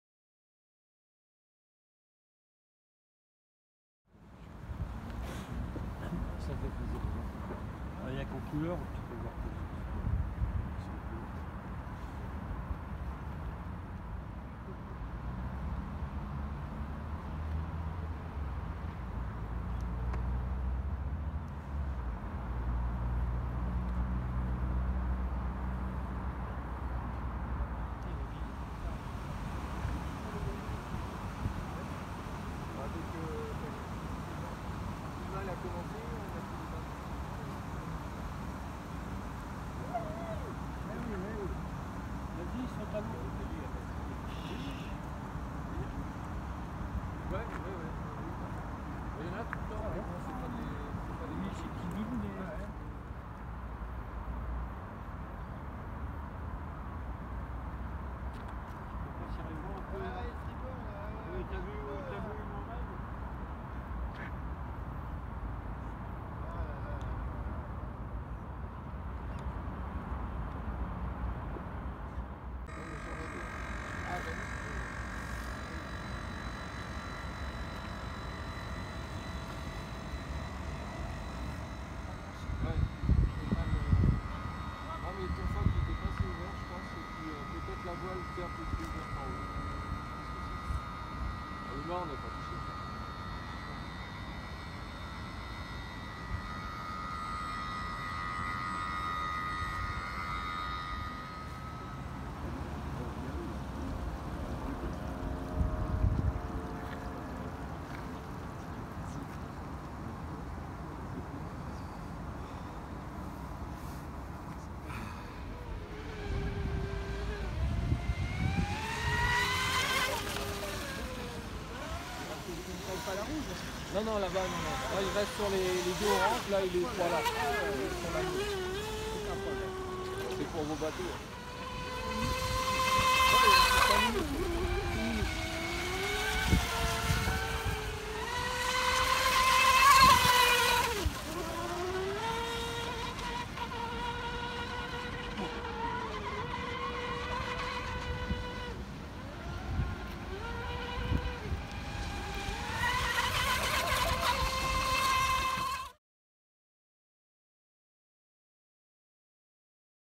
Bateau RC du dimanche 17 décembre 2023
Bateau RC du dimanche matin, pas beaucoup de vent au début, puis vent correct pour les voiliers et les autres bateaux